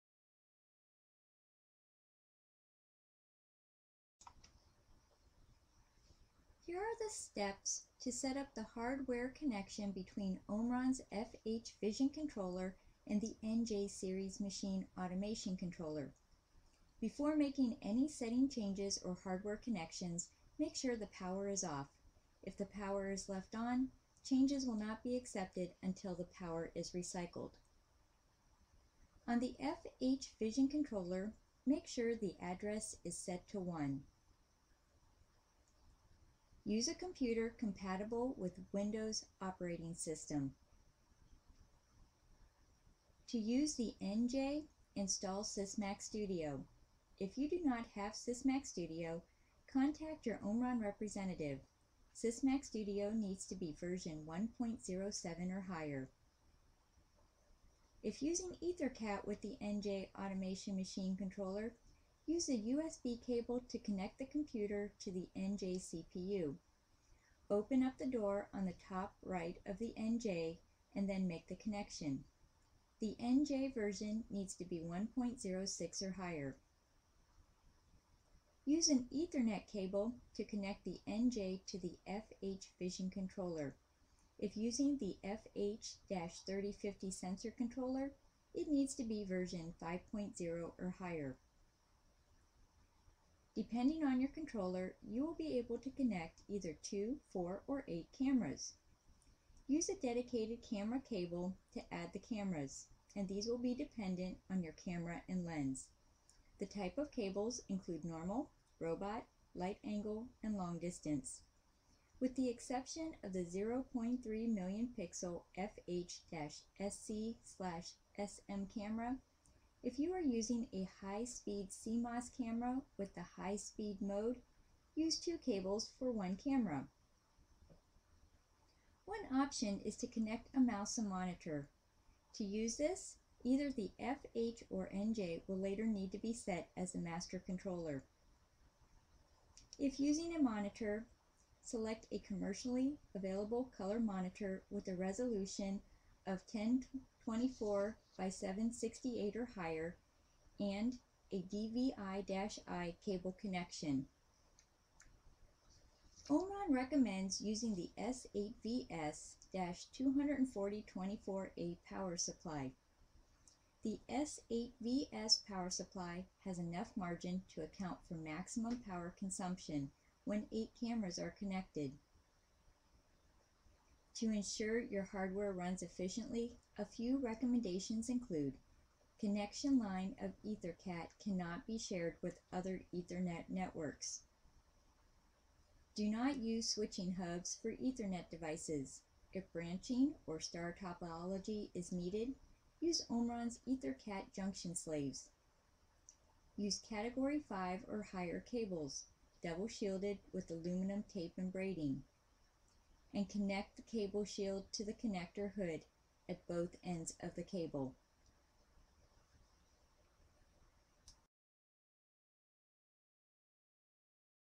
FH Vision Hardware Connection to NJ Machine Automation Controller
How to set-up FH Vision with a NJ Machine Automation Controller using Sysmac Studio - Omron Quick Tip Video